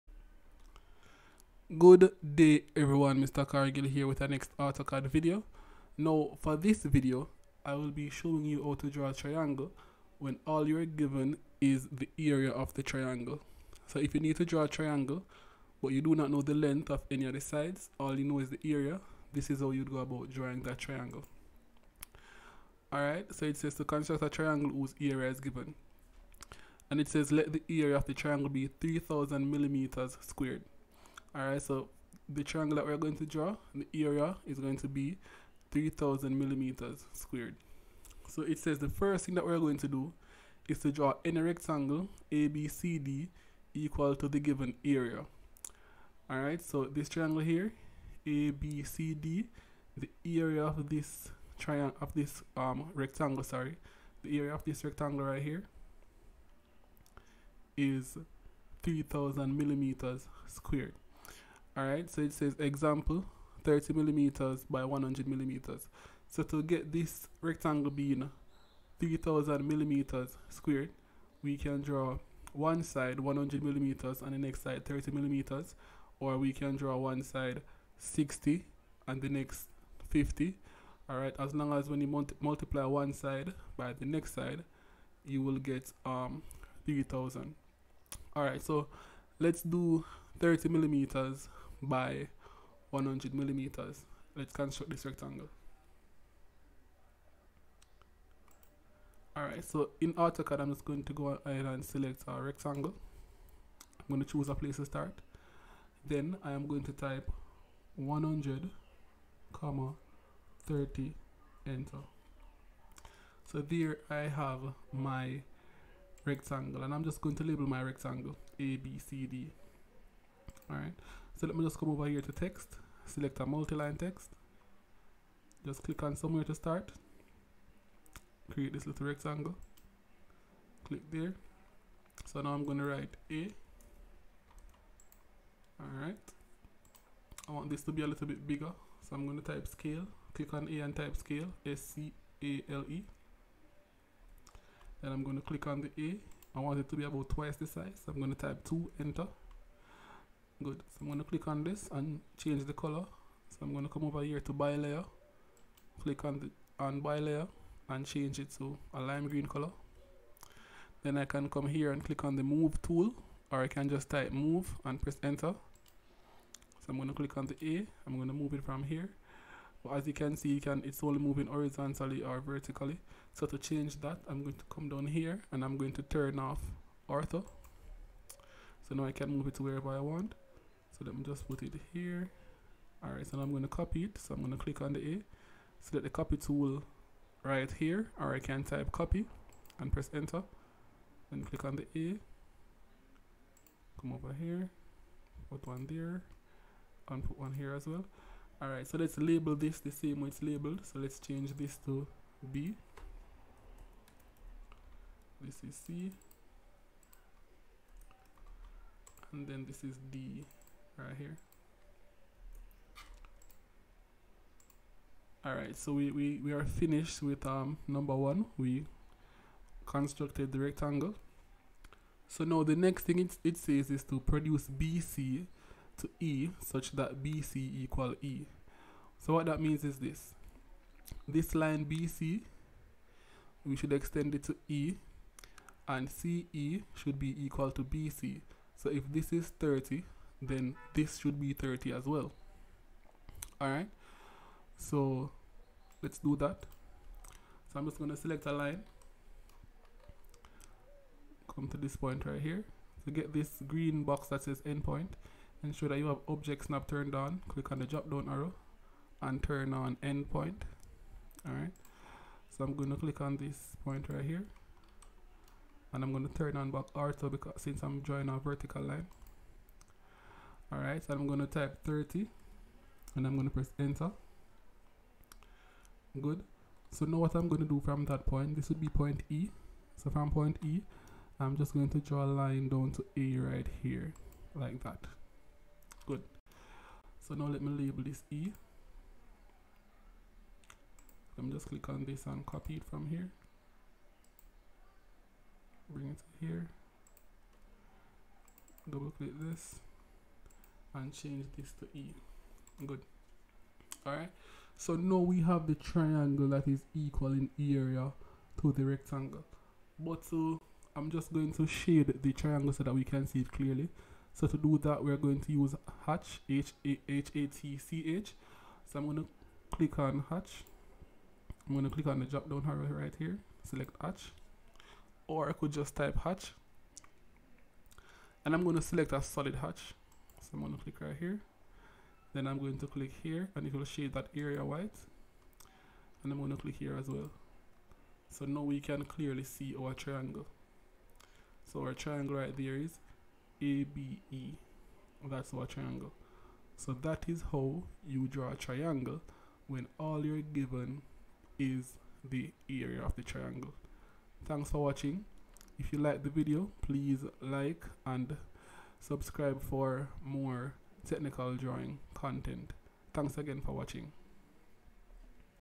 How to draw a triangle given just the area
This video explains how to draw a triangle when you are only given the area. This is a drawing from the topic "plane figures of equal area".
If you are interested in one on one tutoring you can head over to
and book a session with me.